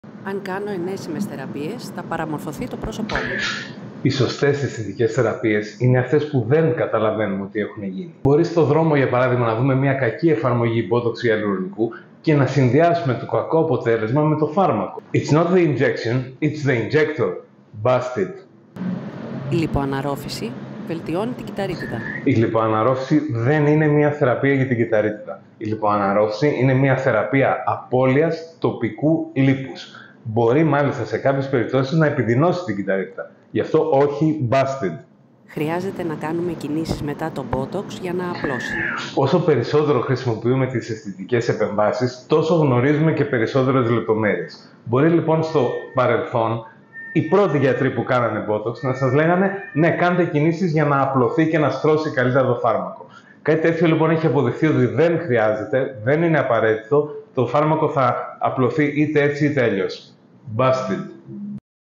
Myth Busters part 2
Myth Busters, Part 2. Με τόση πολύ πληροφορία που δεχόμαστε  καθημερινά από τα social media γύρω από τις αισθητικές θεραπείες είναι πλέον αρκετά δύσκολο να ξεχωρίσουμε τι είναι αλήθεια και το μύθος.

Θα δοκιμάσω να απαντήσω σύντομα και περιεκτικά σε κάποιους από αυτούς και θα χαρώ πολύ να θέσετε τις ερωτήσεις σας στα σχόλια γύρω  από αυτά που έχετε ακούσει και μπορεί να σας προβληματίζουν, για να απαντήσω και σε αυτές!